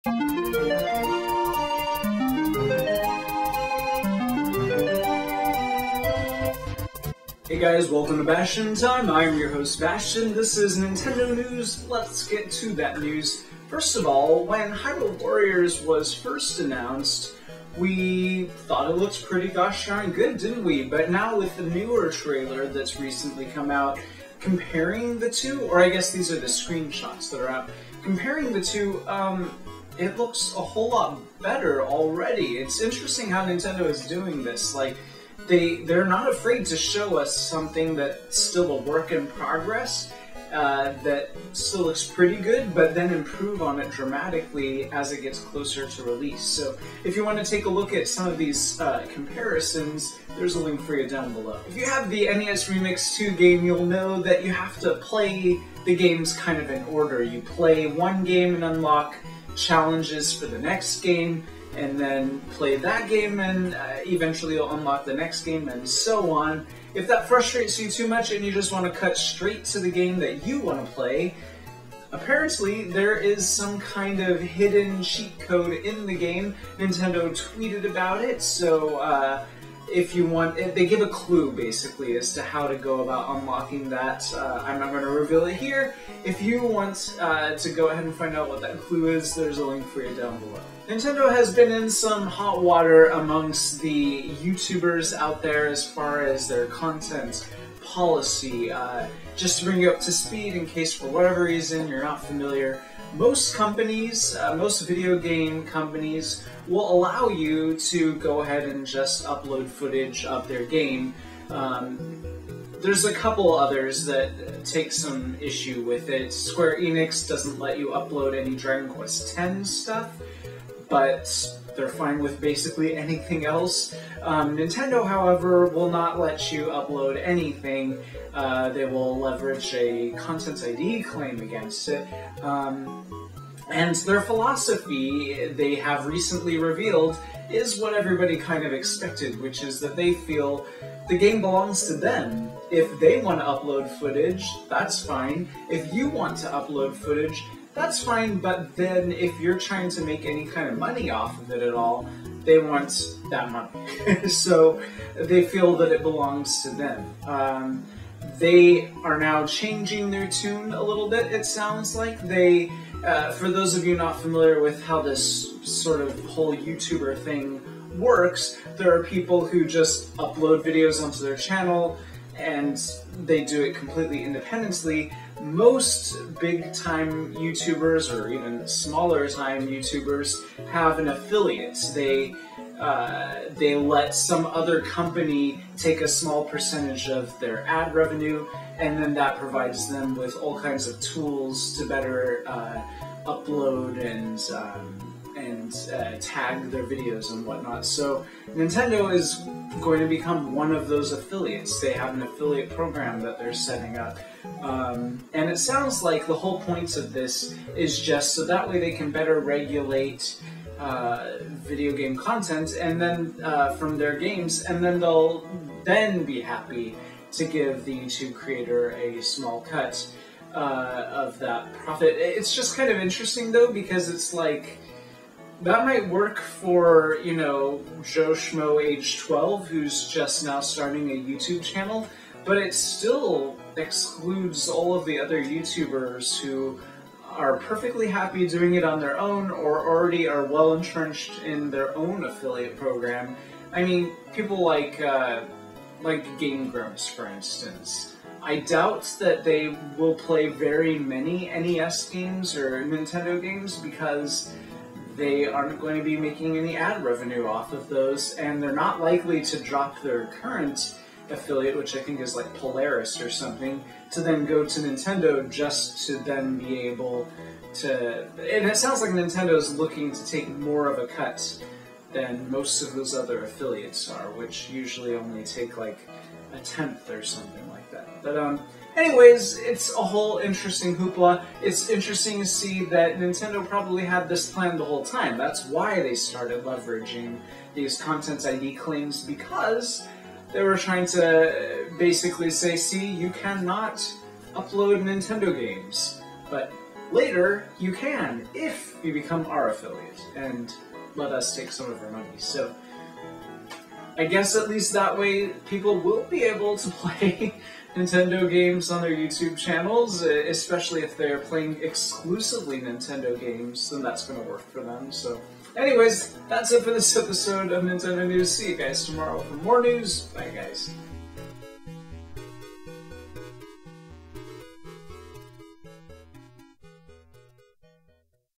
Nintendo News: Nintendo Planning YouTube Affiliate Program
Hyrule Warriors - original reveal vs. recent screens

Nintendo Planning YouTube Affiliate Program to Split Revenue with Youtubers